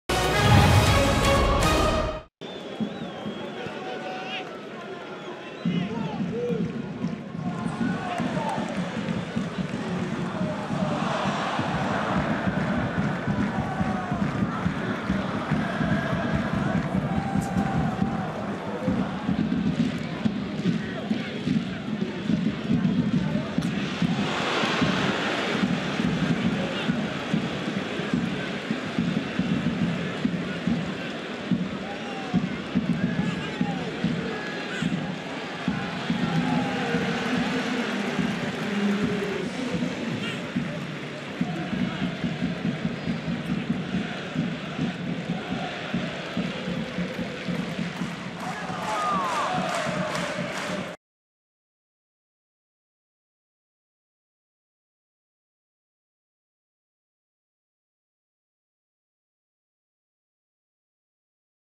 Goal Maxwel CORNET (89') / Nîmes Olympique - Olympique Lyonnais (2-3) (NIMES-OL) / 2018-19
Nîmes Olympique - Olympique Lyonnais ( 2-3) Maxwel CORNET (89') goal. Nîmes Olympique vs Olympique Lyonnais Goals in video

Ligue 1 Conforama - Season 2018/2019 - Week 38 Stade des Costières - Friday 24 May 2019

Goals : Maxwel CORNET (6' - Olympique Lyonnais) / Renaud RIPART (11' - Nîmes Olympique) / Antonin BOBICHON (45' +3 - Nîmes Olympique) / Maxwel CORNET (89' - Olympique Lyonnais) / Tanguy NDOMBELE (90' +1 - Olympique Lyonnais)